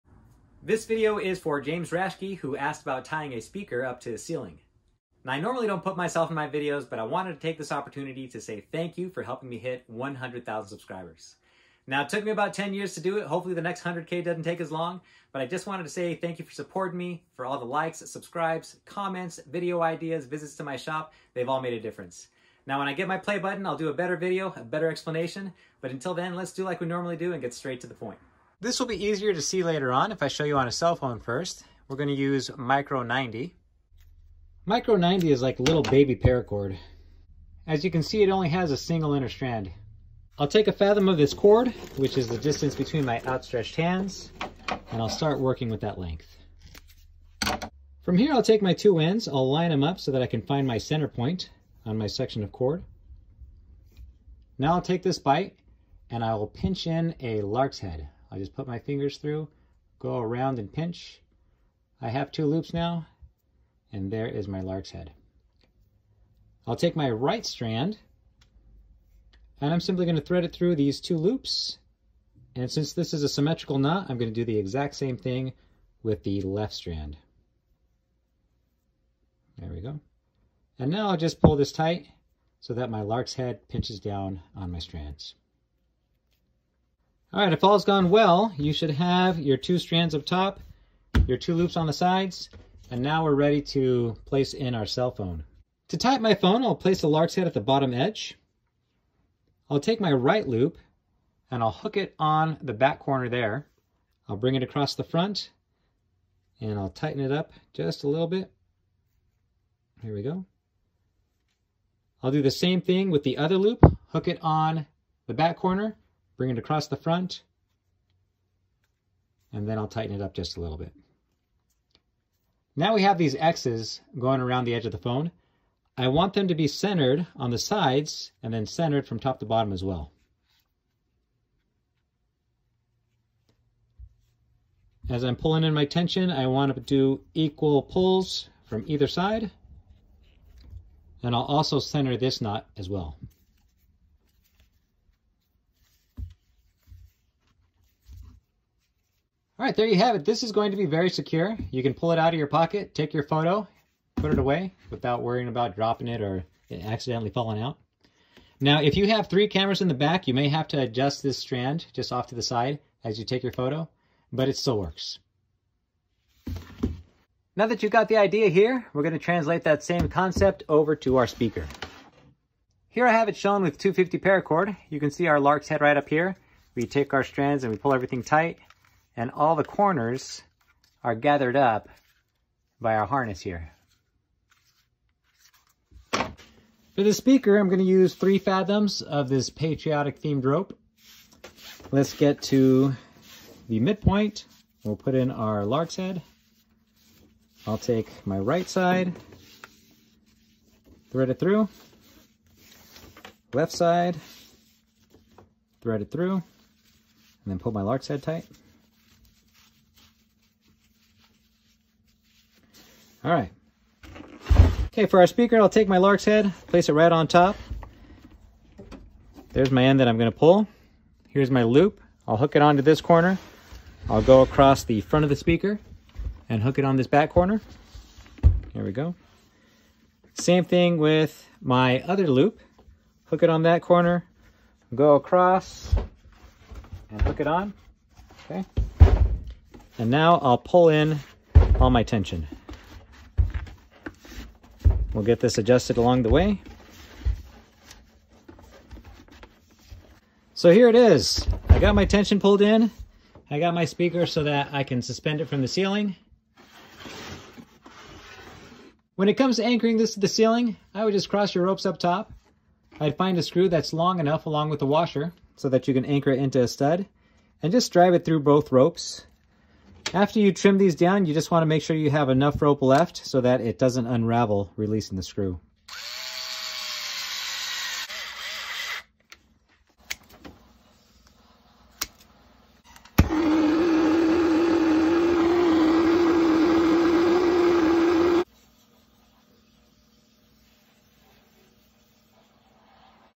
THANK YOU! - A Better Cell Phone Knot. For Speakers too.
Another video is coming once I can show you my silver play button. James asked about tying a speaker to his ceiling. This knot works for any rectangle or cube shaped item. You could also use it for an ID badge.